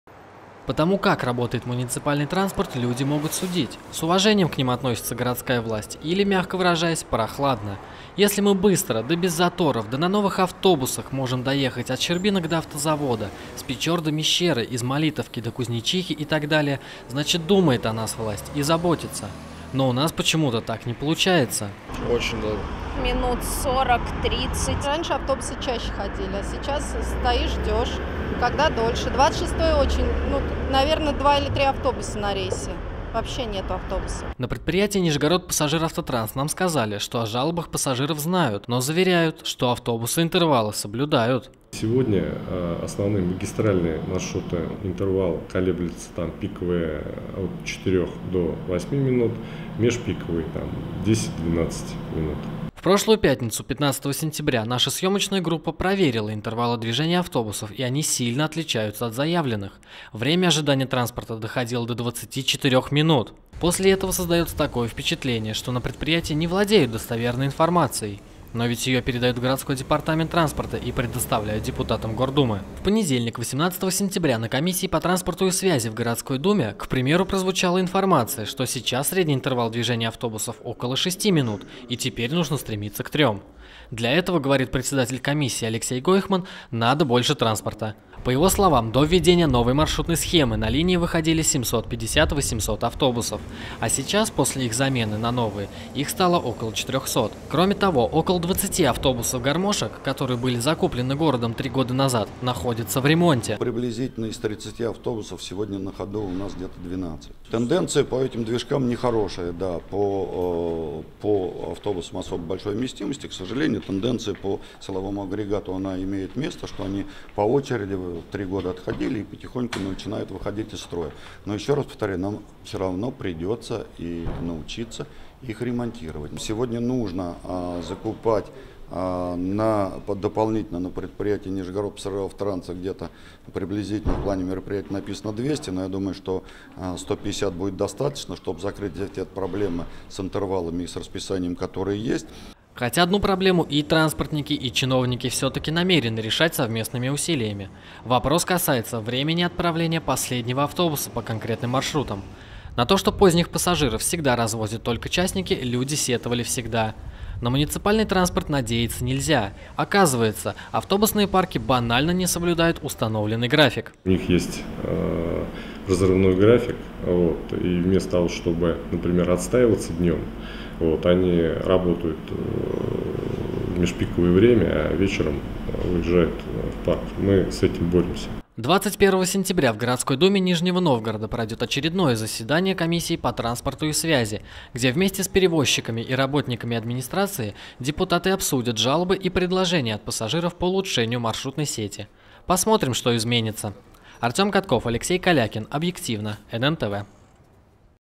Мы продолжаем следить за транспортной ситуацией в Нижнем Новгороде
Горожане по-прежнему жалуются на долгое ожидание муниципальных автобусов. Вместо заявленных шести минут бывает, приходится стоять на остановке до получаса. В городской администрации объясняют это почти двукратным сокращением числа автобусов. Старые списали, а новых купили недостаточно. Эта тема вновь стала центральной и на заседании профильной комиссии Городской Думы.